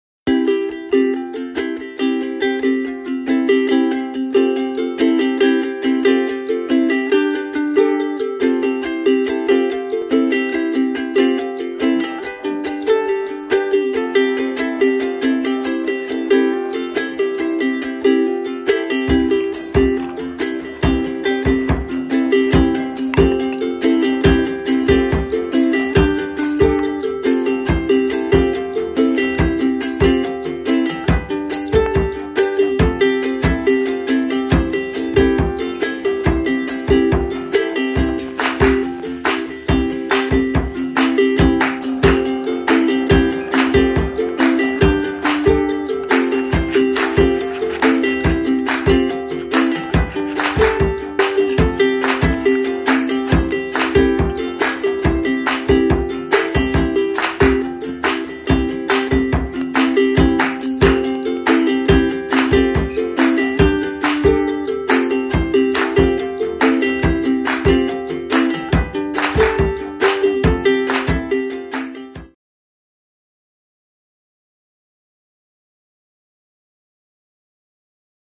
Jhinge Bata recipe. Ridge gourd recipe. Simple but tasty recipe
Uniquely cooked jhinge bata recipe. People who don't like this vegetable will also love to eat this dish. Its healthy and super delicious... Goes well with hot rice...